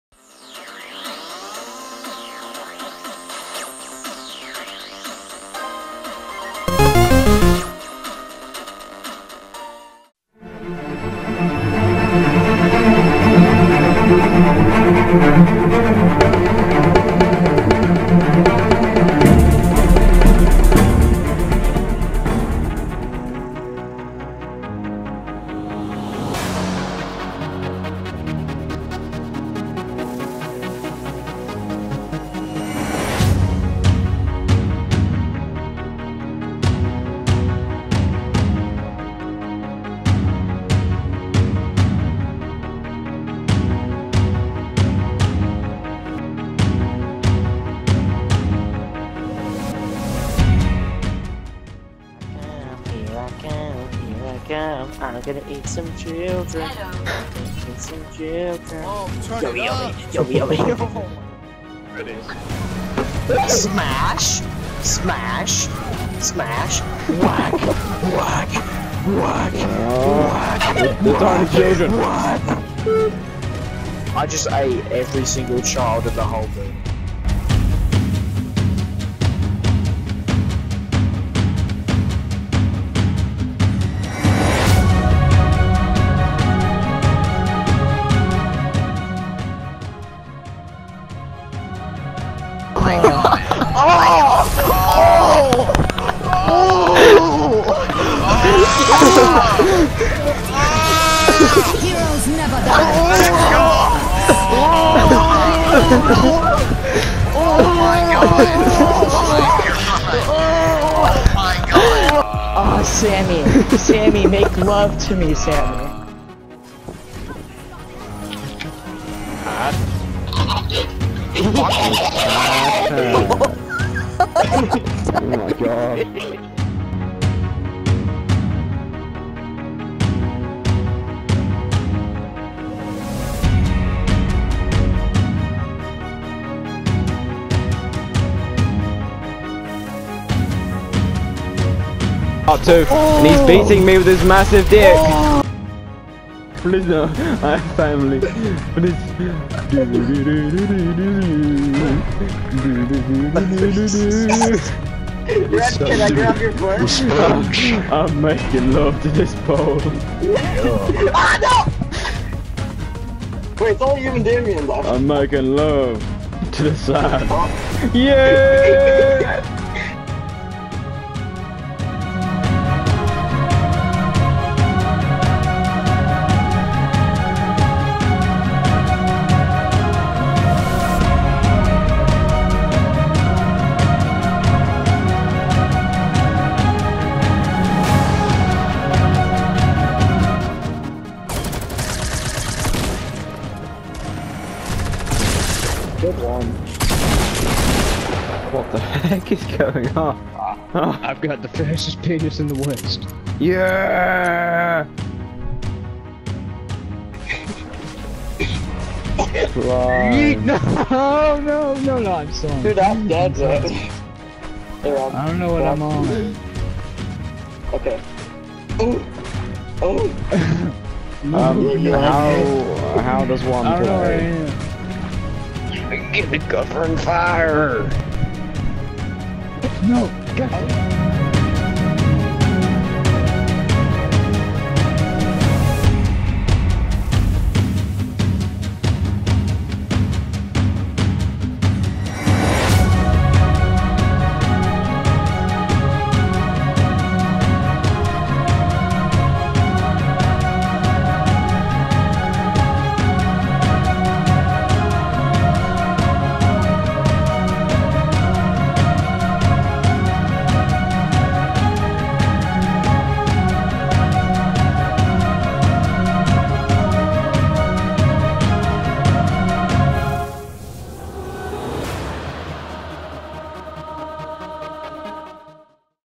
The Originals Intro!
Welcome to the Originals: a group of four Friends who play games and stuff.Go check us all out on Twitch and Youtube links are below.